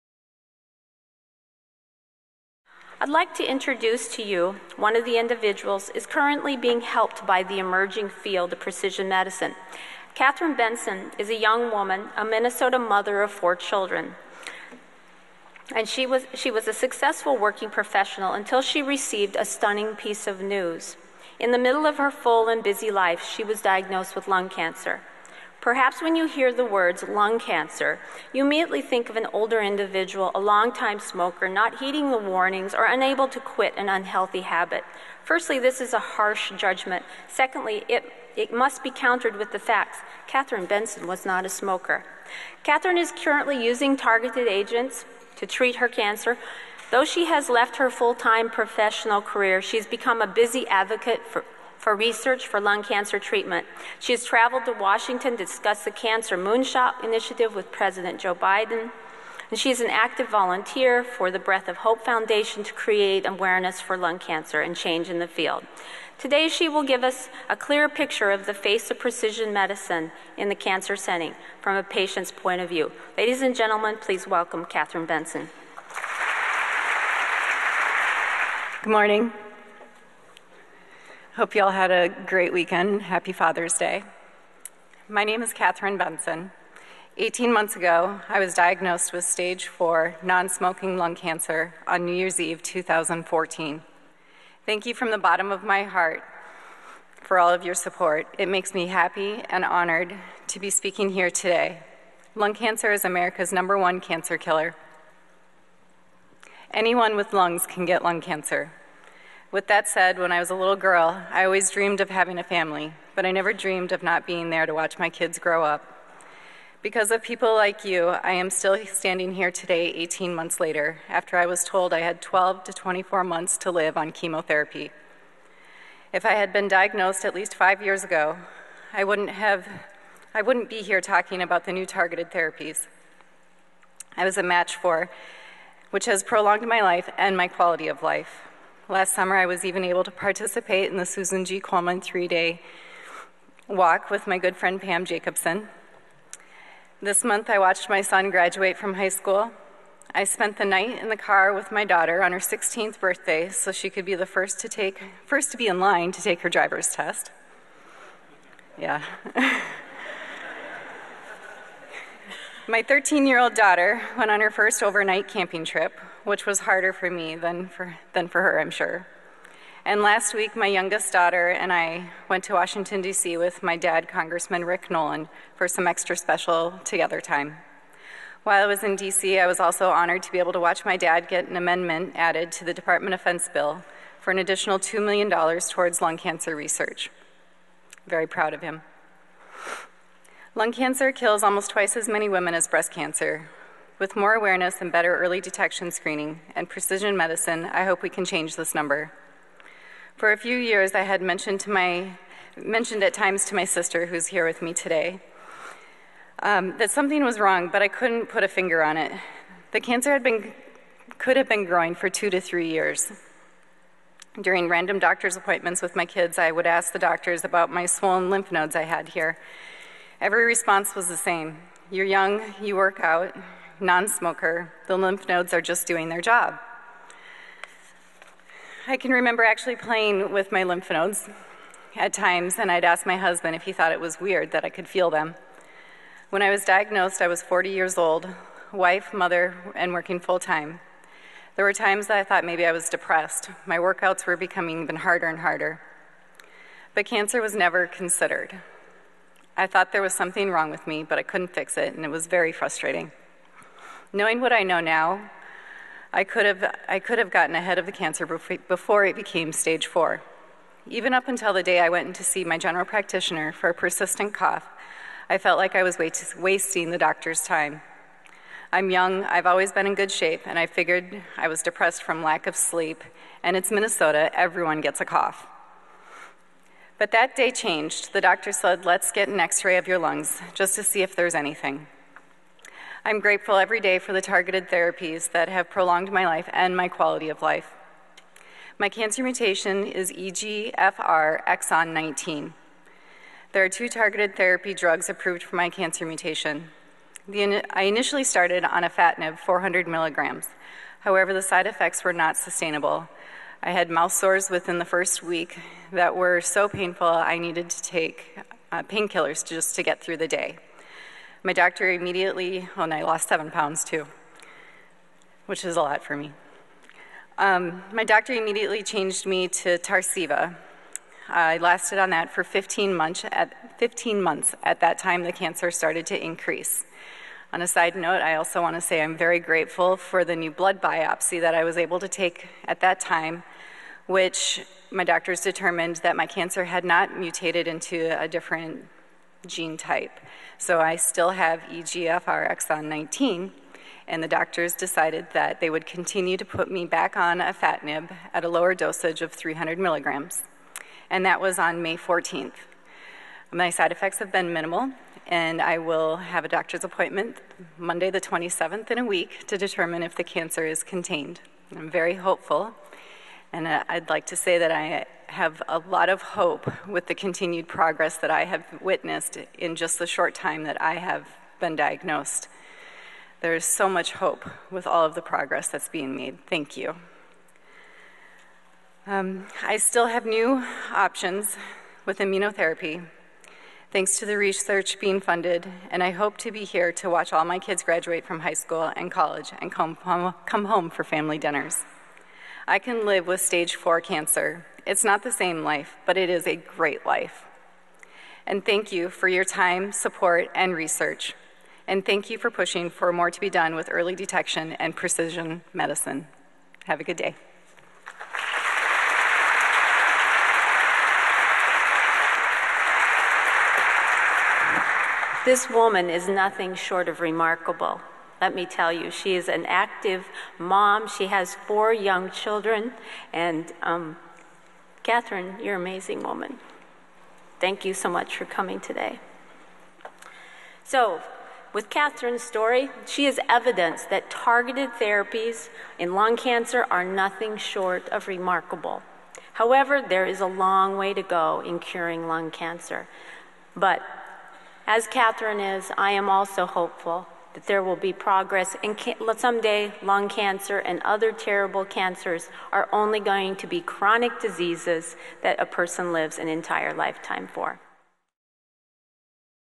03 "Perspectives on Precision Medicine " Katherine Bensen, Precision Medicine Conference 2016
"Perspectives on Precision Medicine from Current Cancer Patient"
Katherine Bensen, BA

The Precision Medicine Conference 2016
Pharmacogenomics: Research To Implementation
Institute of Personalized Medicine (PUMA - IPM)
University of Minnesota